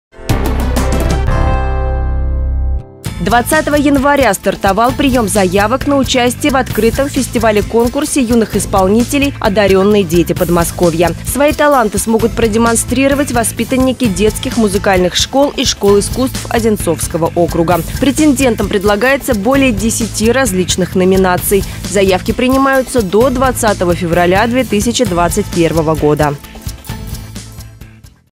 Одарённые дети Подмосковья
20 января стартовал приём заявок на участие в Открытом фестивале-конкурсе юных исполнителей «Одарённые дети Подмосковья».